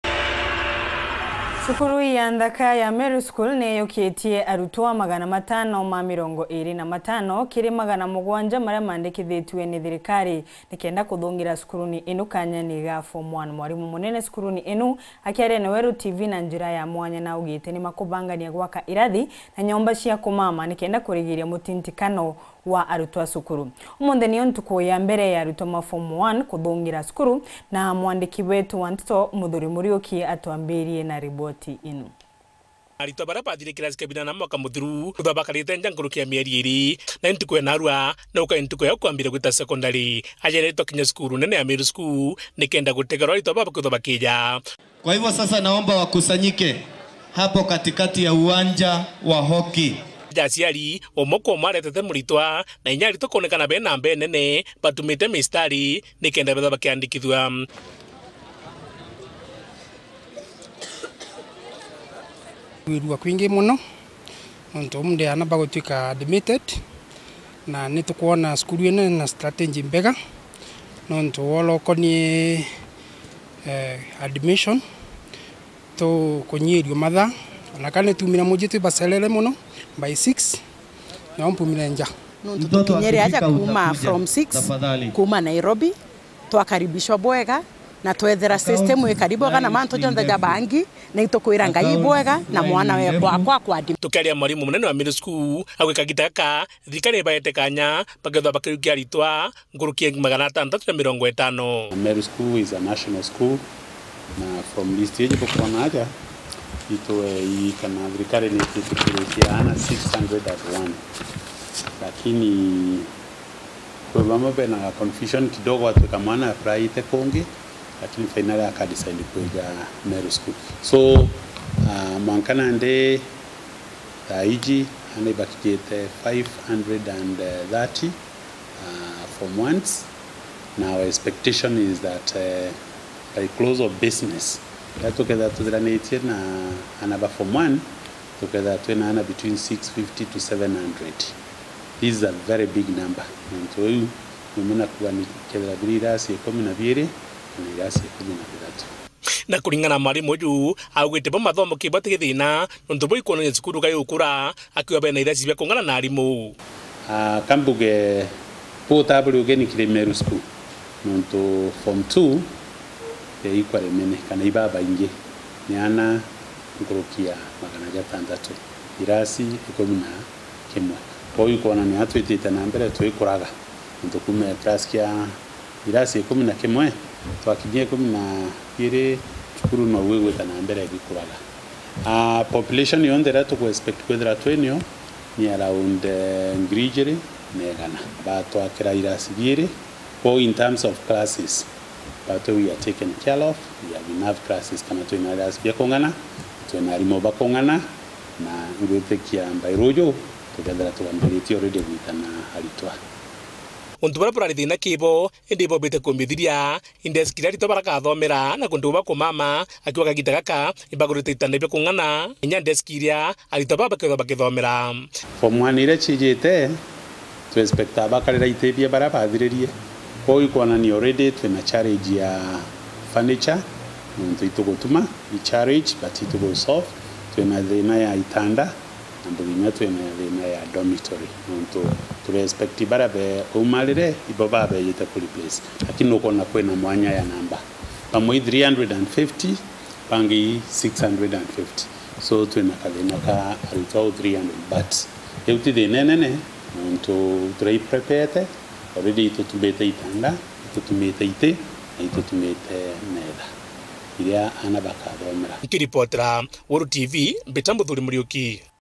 KWAMUKIRA ARITWA BABERU
ARITWA BA FORM 1 KWANJIRIA GUTONYA CUKURU IRIA BARETI
MERU SCHOOL NIAMUKIRITE ARITWA 525/700 NAARUA
NIBAGWAKA IRACI BINGI KENDA KURIGIRIA MUTINTIKANO